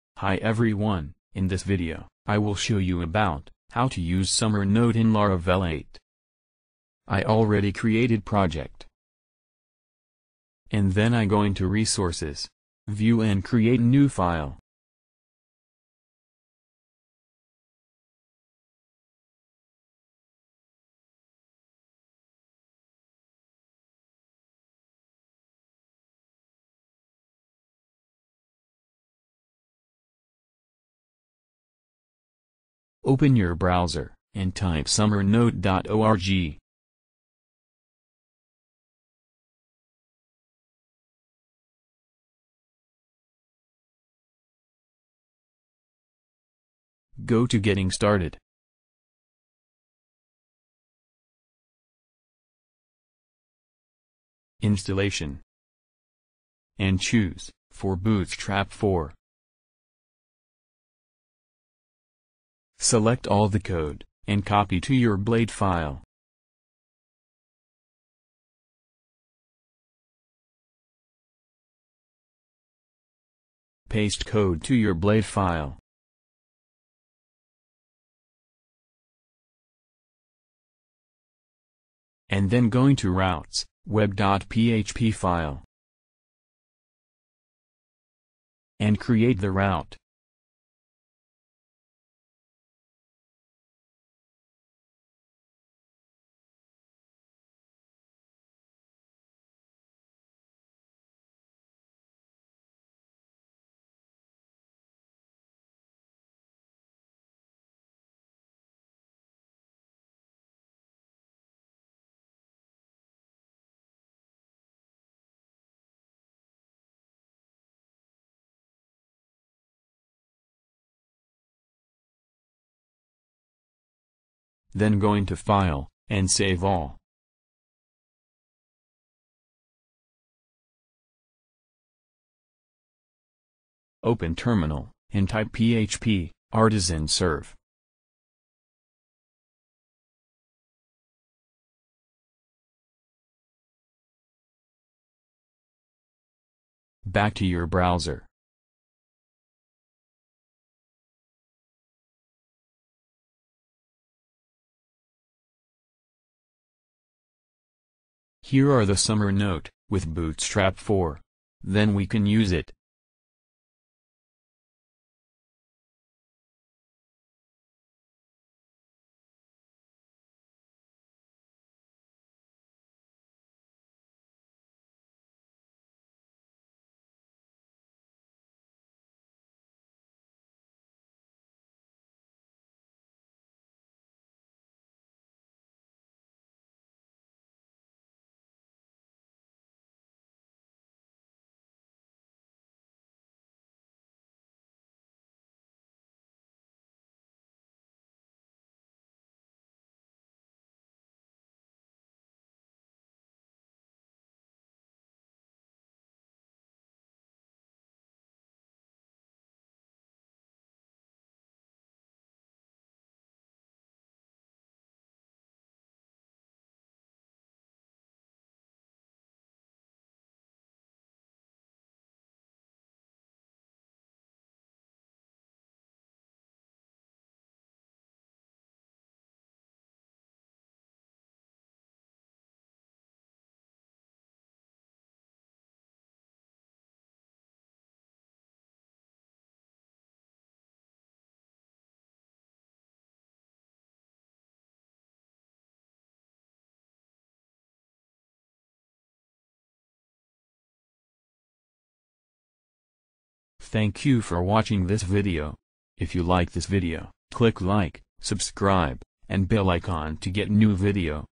How to use summernote in laravel 8
How to use summernote wysiwyg editor with bootstrap 4 in laravel 8